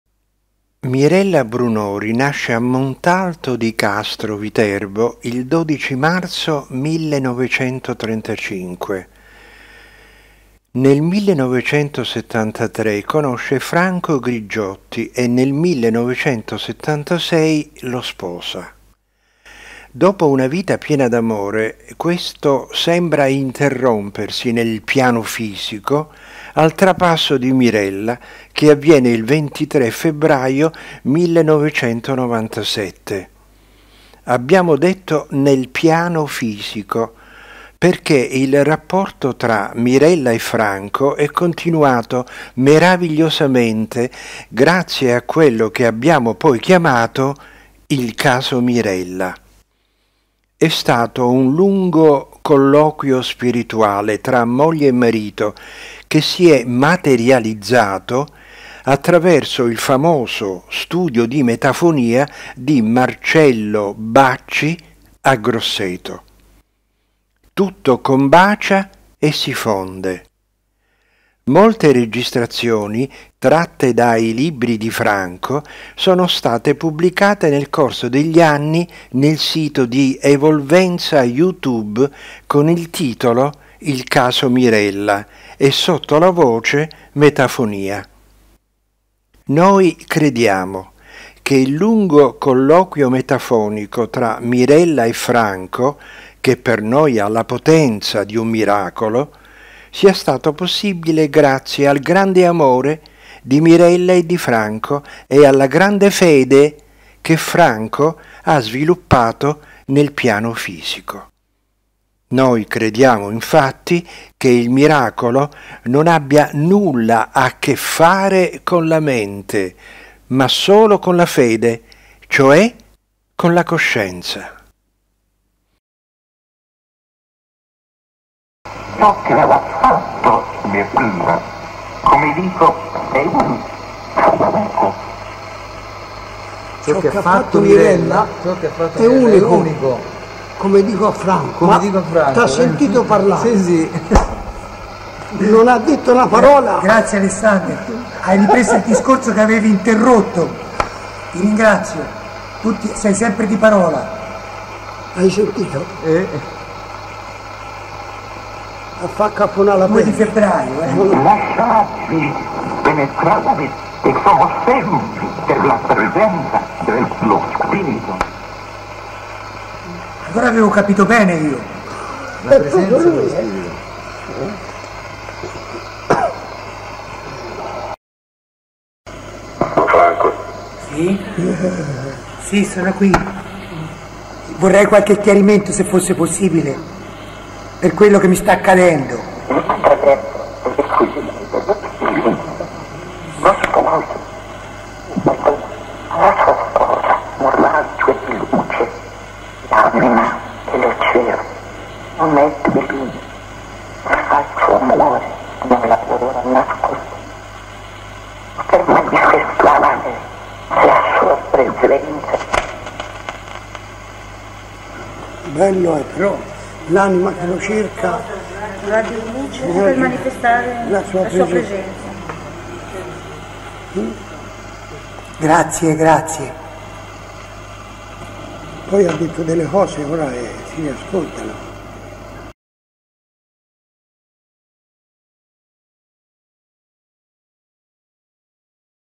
Evolvenza Per la presenza del suo spirito Metafonia Il caso Mirella
- La metafonia è il primo segnale dell’aldilà.
- Electronic Voice Phenomena is the first signal of the afterlife.
- Los Fenómenos de Voz Electrónica son la primera señal de la vida futura.
- Les phonèmes vocaux électroniques sont le premier signal de l'au-delà.
- Elektronische Sprachphänomene sind das erste Signal des Jenseits.
Evolvenza.it